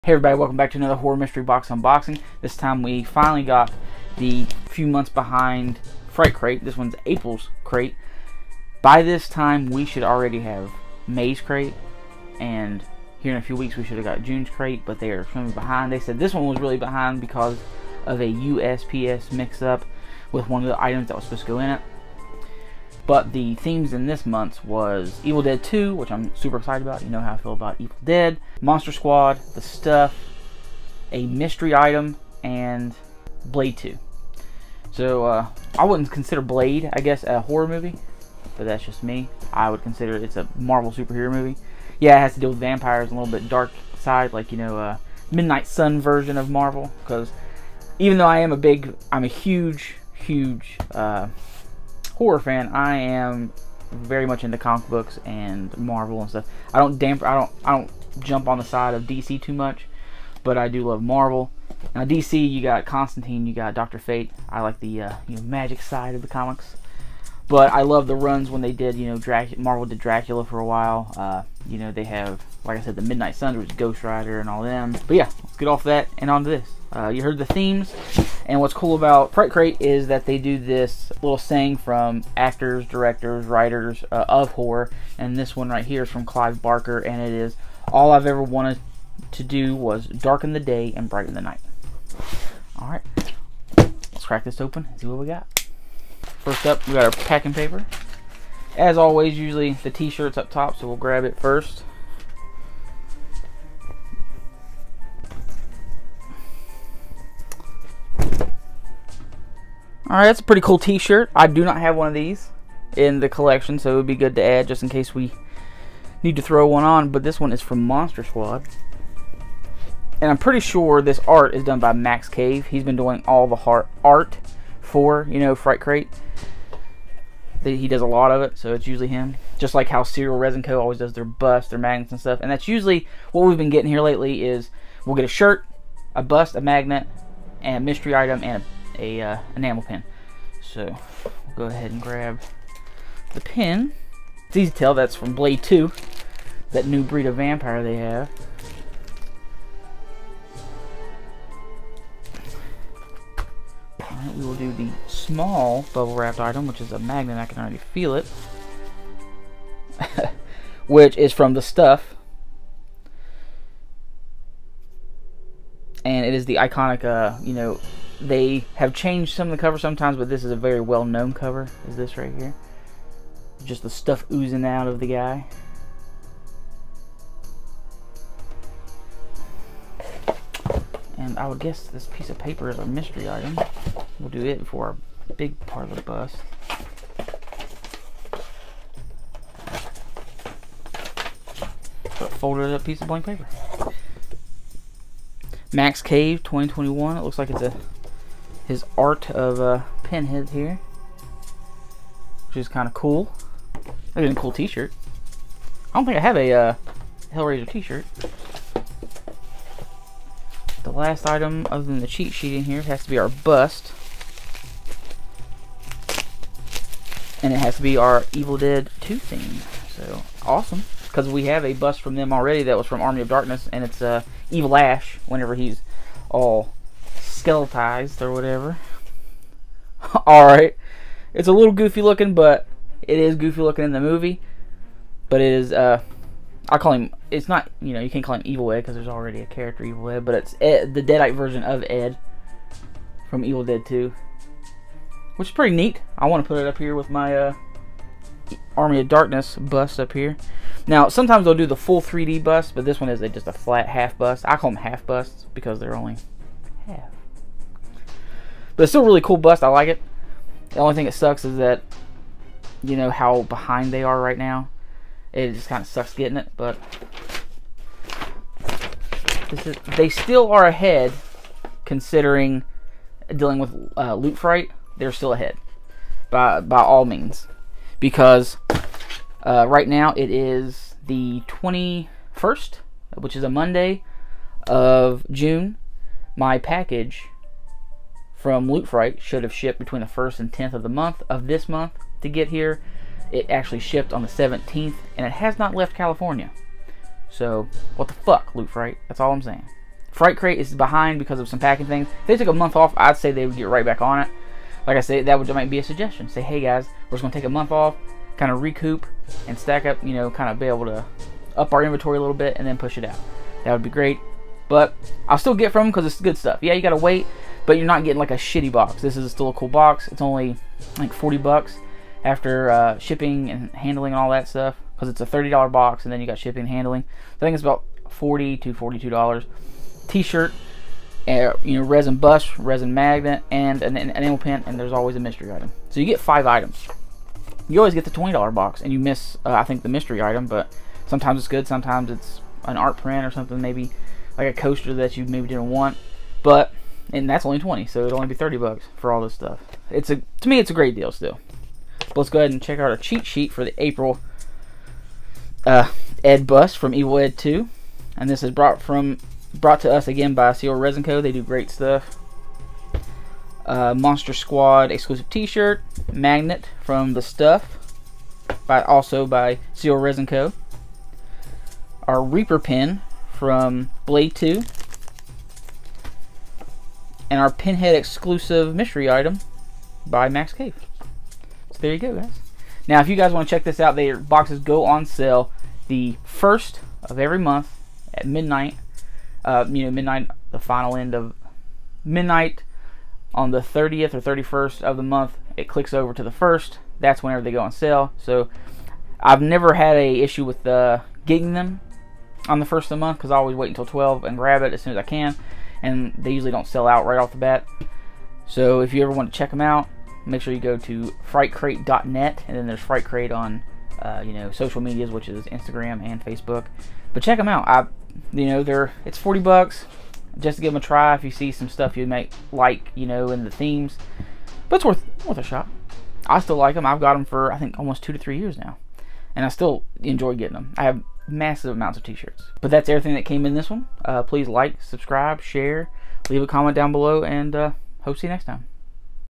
NEW!! Fright Crate (April 2021)
Hi everyone it's Chuck here with another horror mystery box unboxing, this time we have April's Fright Crate.

Inside we have:
The Stuff
Evil Dead 2
Monster Squad
Blade 2
1 mystery theme